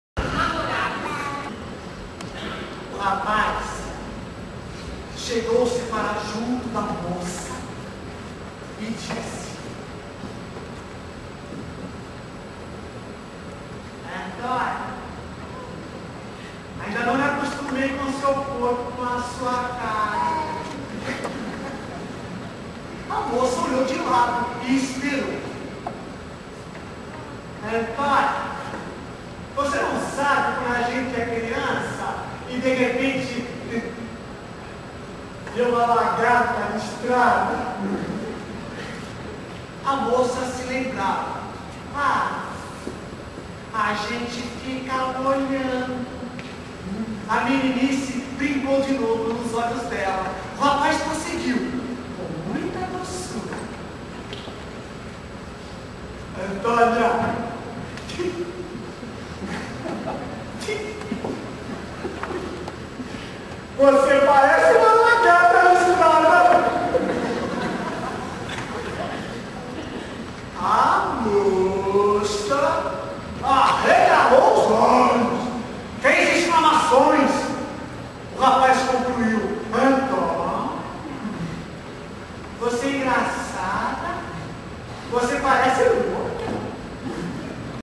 Poema Namorados de Manuel Bandeira Ator e Diretor Luiz Thomas com Suzanne Diaz
Poema Namorados de Bandeira
Tirado do Espetáculo Eu Tomo Alegria
Com 32 poesias de Manuel Bandeira
E trechos contados de sua vida
Assista a peça completa vendo as partes